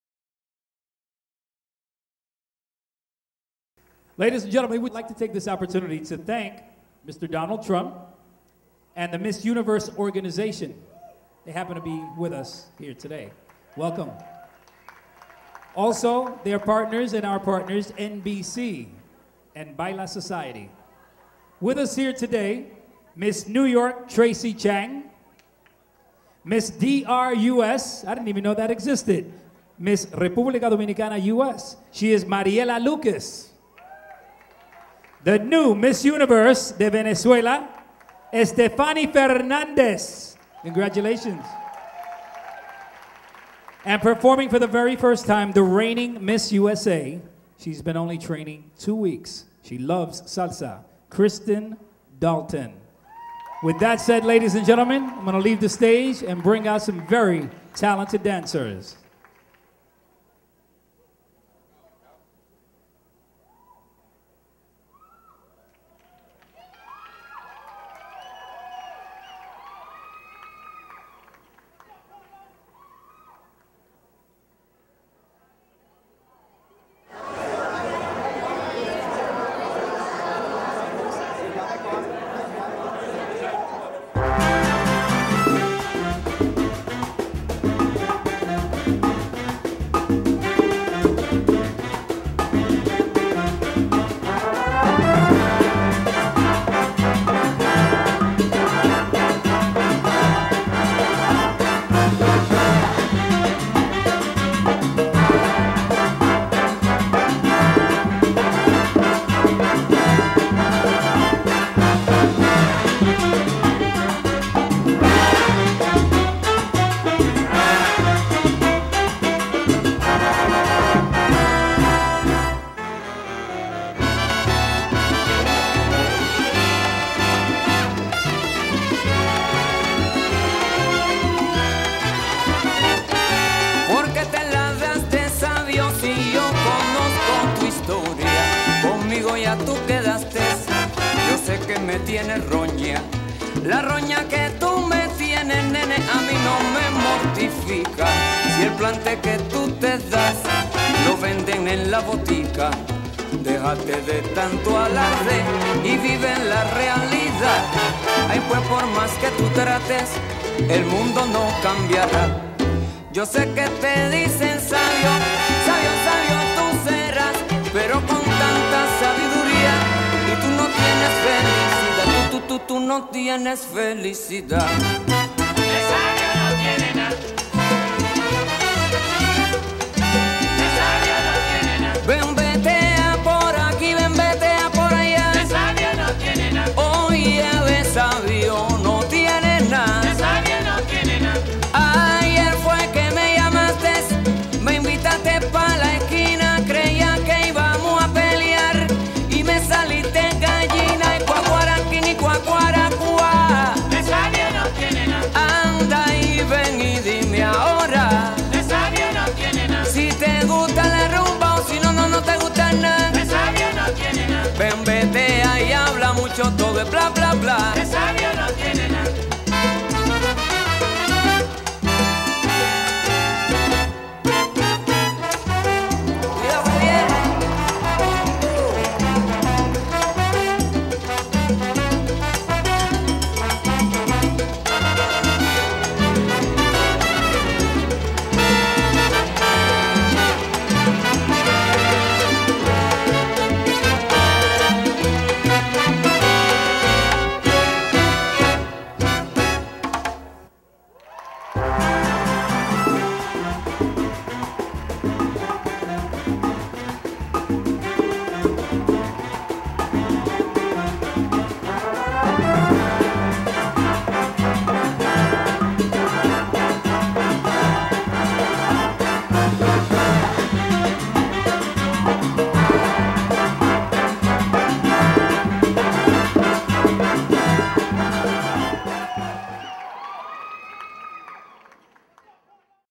NY Salsa Congress 09 BAILA Society (BASo) Performance with Miss USA
BASo performs with Miss USA 2009, Kristen Dalton, at the NYISC 2009, New York, NY